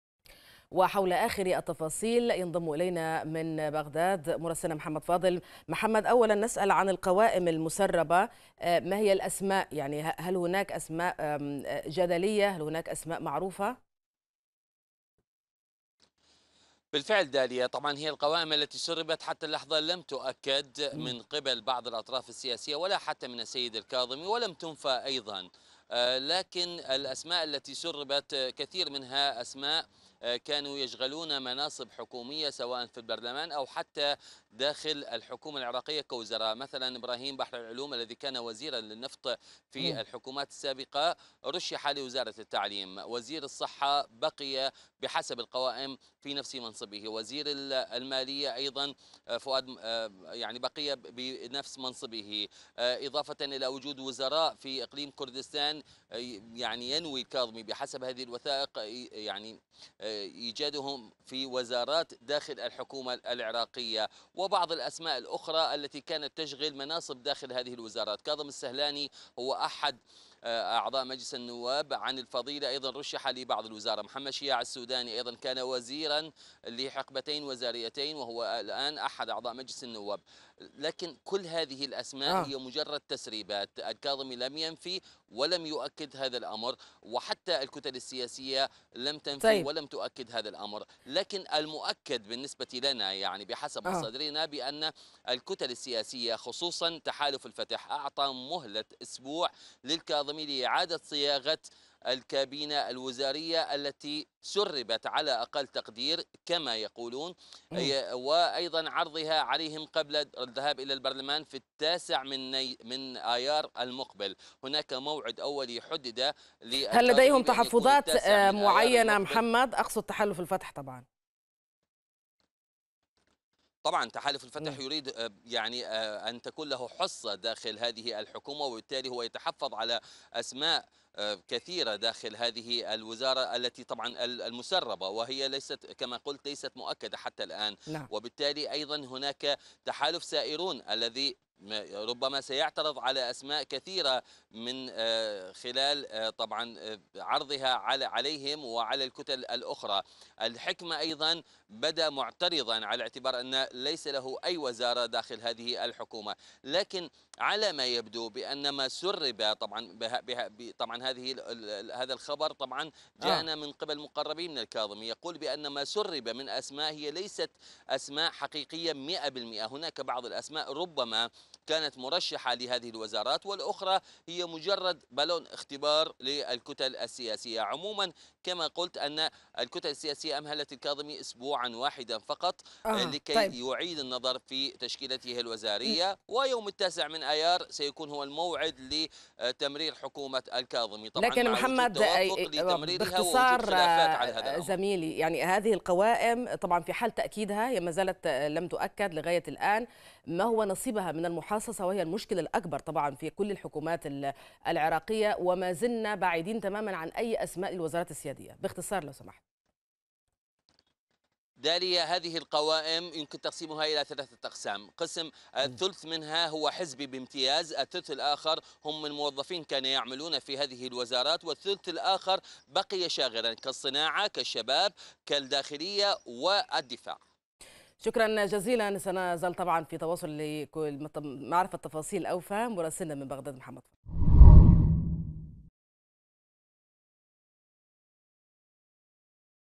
تسريب قوائم وزارية.. نرصد مستجدات التشكيل المرتقب للحكومة العراقية
أفاد مراسل الغد بتسريب بعض المواقع الصحفية أربع قوائم للتشكيلة المرتقبة لحكومة رئيس الوزراء المكلف مصطفى الكاظمي وذلك بالتزامن مع عقده اجتماعا مع عدد من قادة الكتل السياسية، لبحث الأسماء المرشحة للحكومة الجديدة.

وكشف مصدر مقرب من الكاظمي ، أن الخلافات تركزت خلال الاجتماع على حقائب الوزارات السيادية ،وهي الدفاع والداخلية والنفط والمالية.

المزيد من التفاصيل يتحدث مراسل قناة الغد من بغداد، محمد فاضل، عبر النشرة الإخبارية